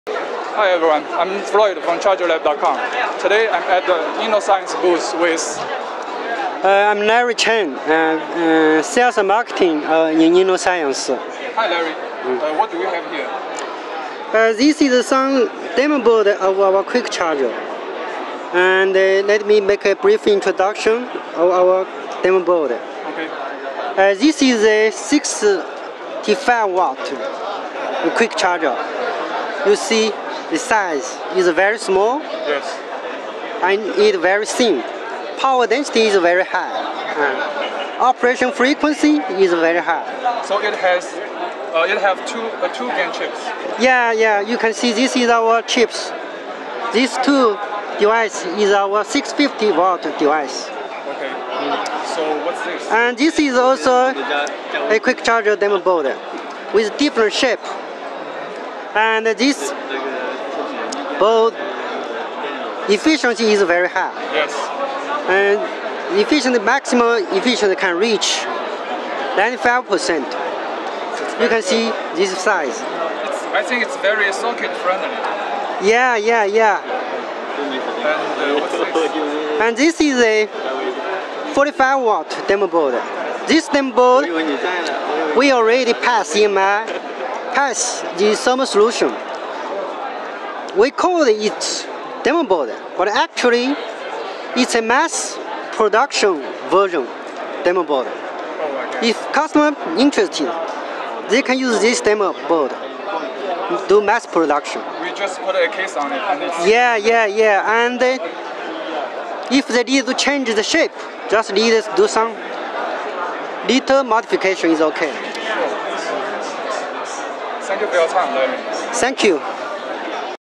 Innoscience Showcases GaN Charger Solutions at USB-PD Asia 2019
Larry Cheng, Vice President of Marketing at Innoscience, introduced Innoscience’s innoGaN charger solutions at 2019 (Autumn) USB PD & Type-C Asia Expo in Shenzhen, China, on August 23, 2019.